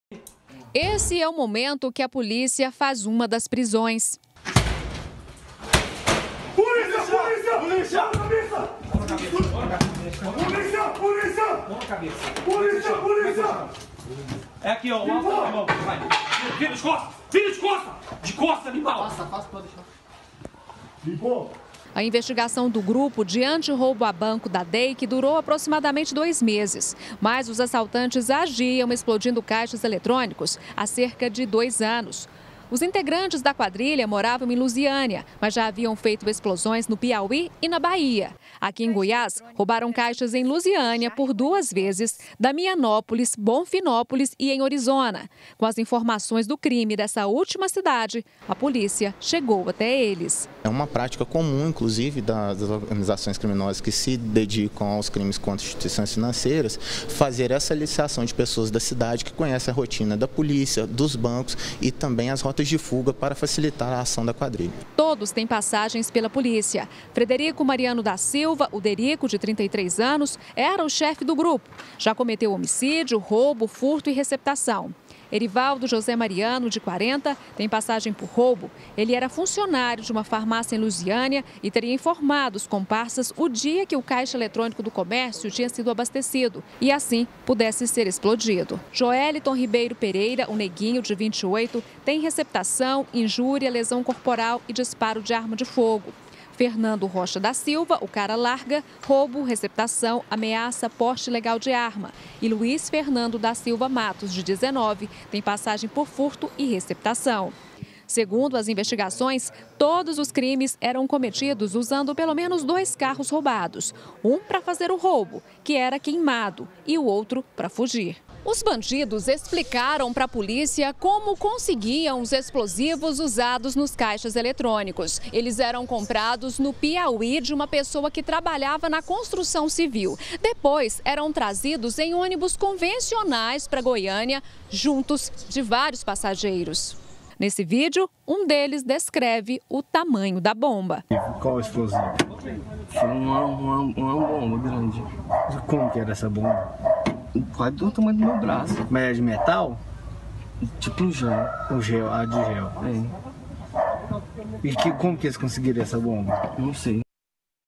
BG - Presa quadrilha que explodia bancos - 15-01-2018
Polícia apresenta quadrilha que roubava bancos nos estados de Goiás, Piauí e Bahia. os explosivos usados pra explodir os caixas eram trazidos de ônibus do Piauí. O próximo alvo seria uma agencia da caixa econômica federal na cidade de Novo Gama.